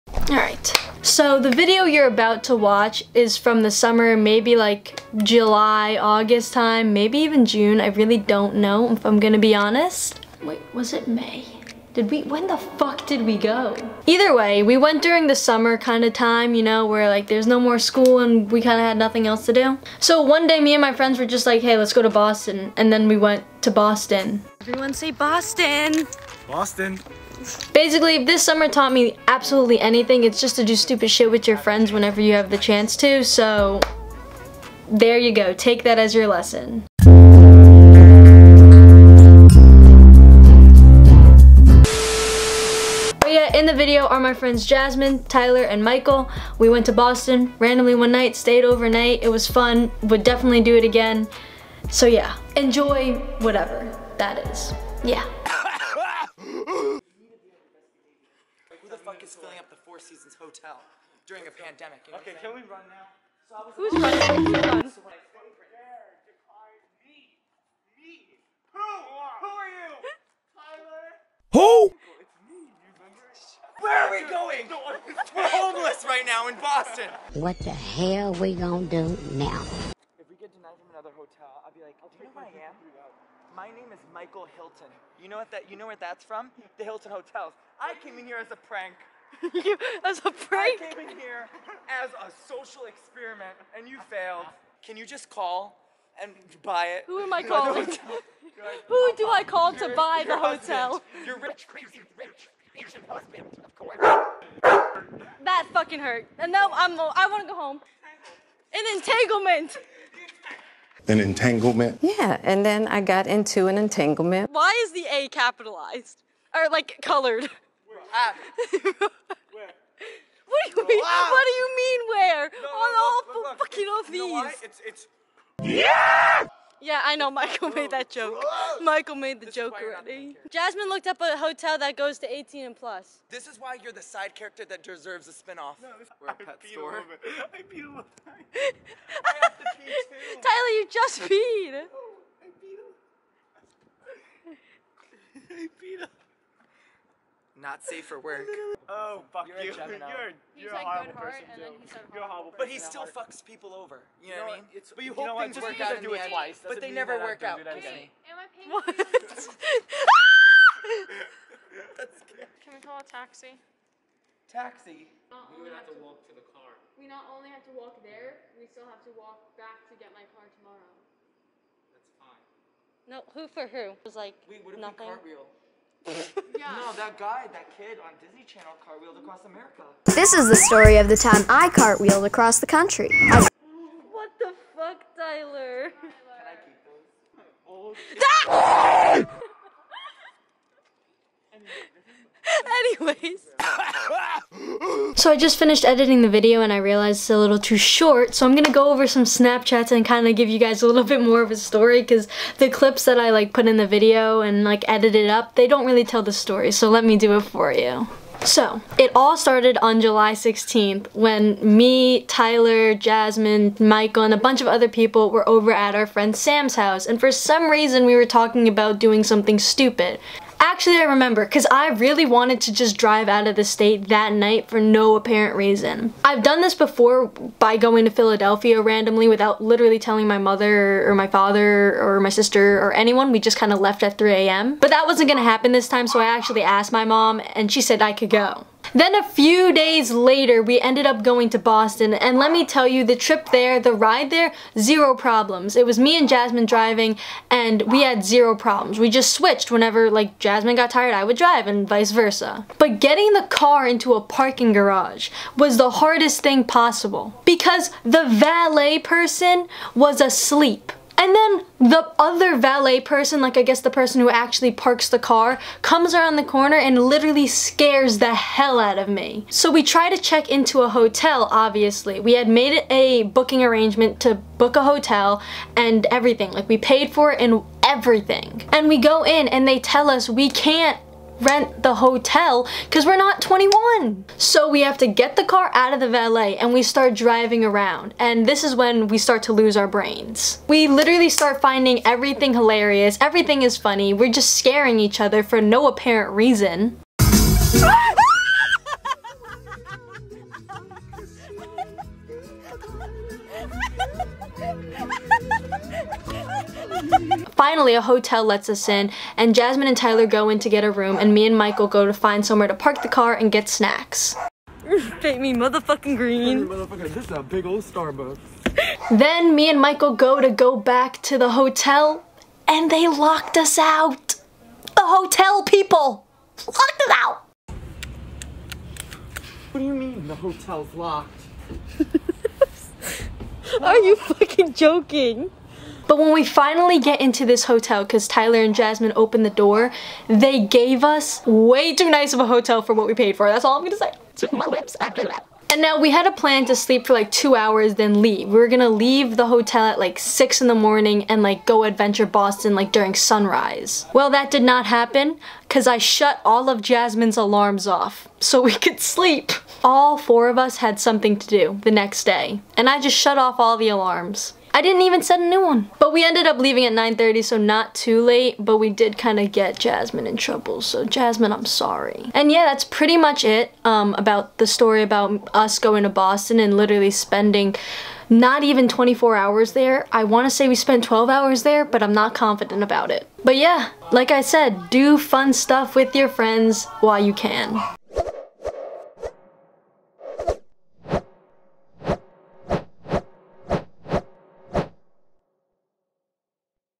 WE WENT HOMELESS IN BOSTON!
My friends and I decided to go to Boston randomly one night during the summer and it didn't go as smooth as we expected...